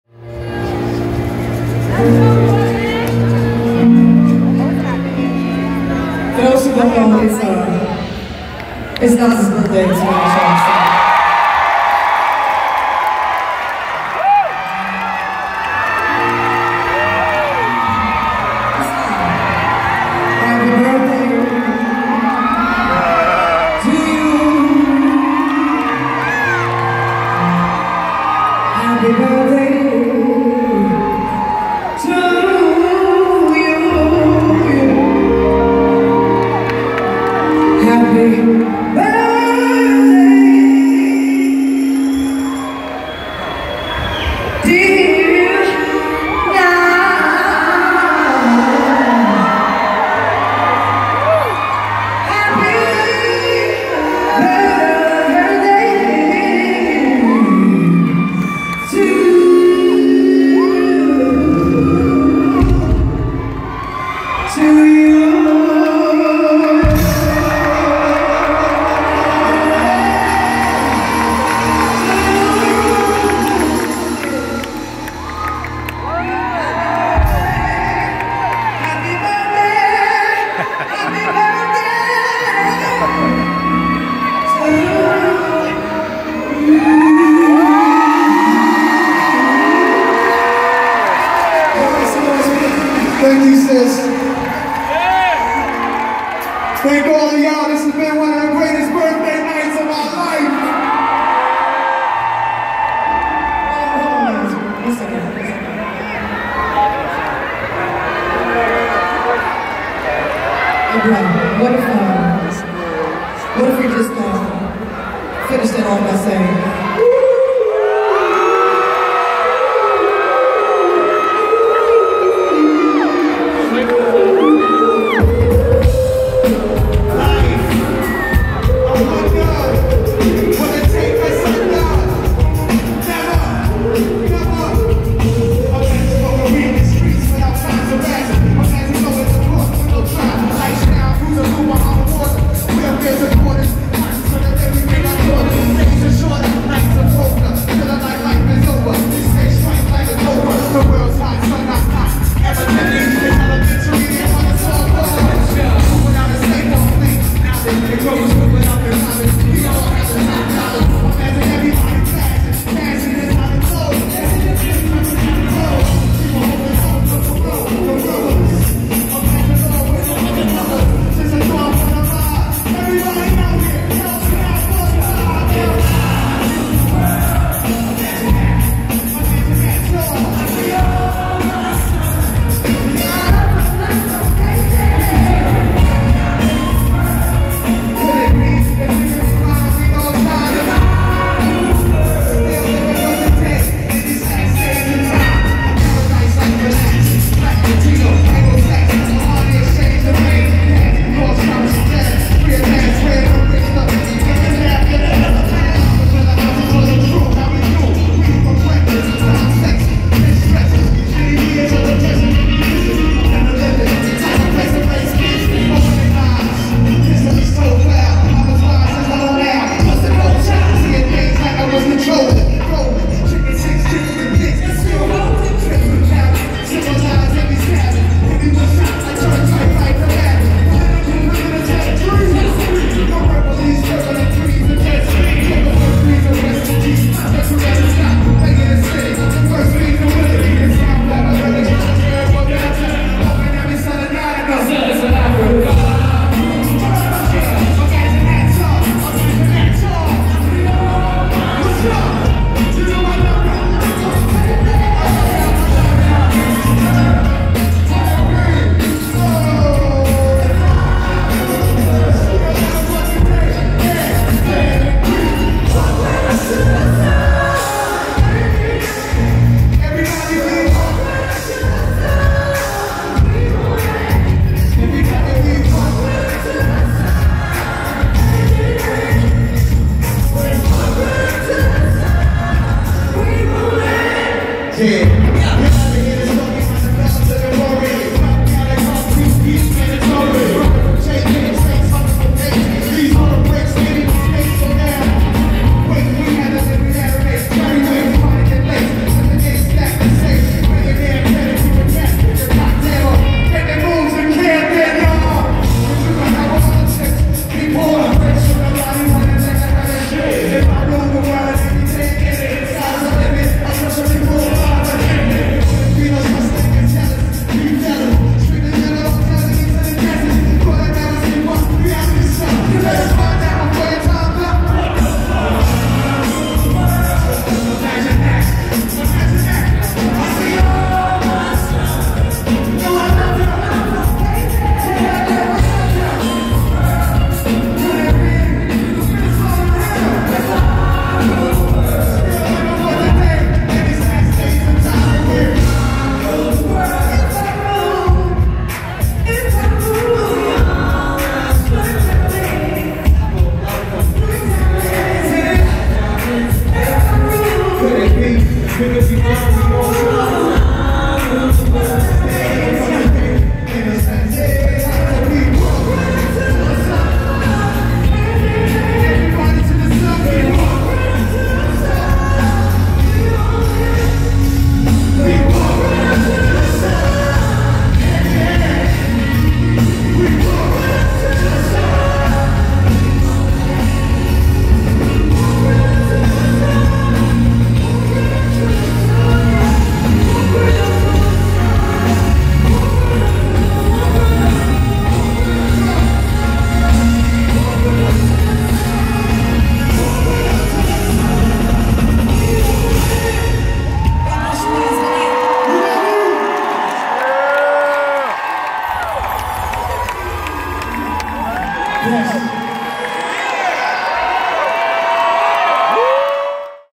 NAS X LAURYN HILL LIVE
Thu, Sep 14, 2017 BB&T Pavilion Camden, NJ
Nas X Lauryn Hill Concert on his birthday. Great way to end such an energetic powerful performances by both artist.
filmed on my GalaxyS8 from the 4th row...